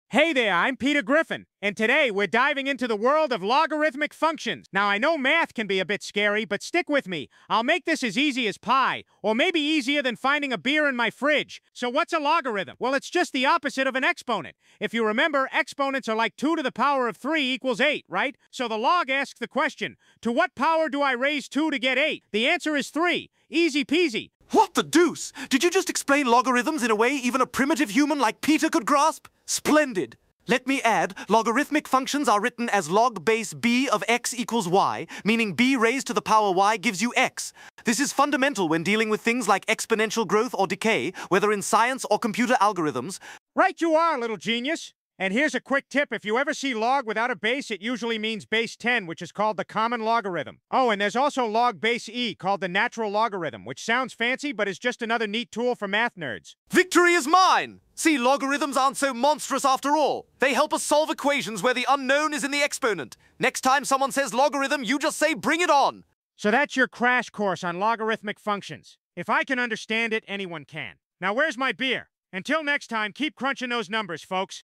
Basic Logarithmic functions for beginners
What happens when Peter Griffin tries to teach Stewie what a logarithm is? Absolute chaos... and a little bit of learning. 😅

In this absurdly funny crossover of Family Guy and math class, Peter attempts to explain what logarithms are—yes, that confusing "log" thing you see in math problems. Stewie's not impressed… until Peter drops some unexpected wisdom.

🔥 "It’s like the opposite of exponents, Stewie. Duh."
👶 “Good God, what have I done to deserve this imbecile as a tutor?”

Expect math jokes, confusion, and classic Griffin nonsense—all while accidentally making logs make sense. Kind of.

💥 Watch, laugh, and actually learn something. Maybe.

👍 LIKE for more cartoon math chaos
📌 COMMENT your favorite quote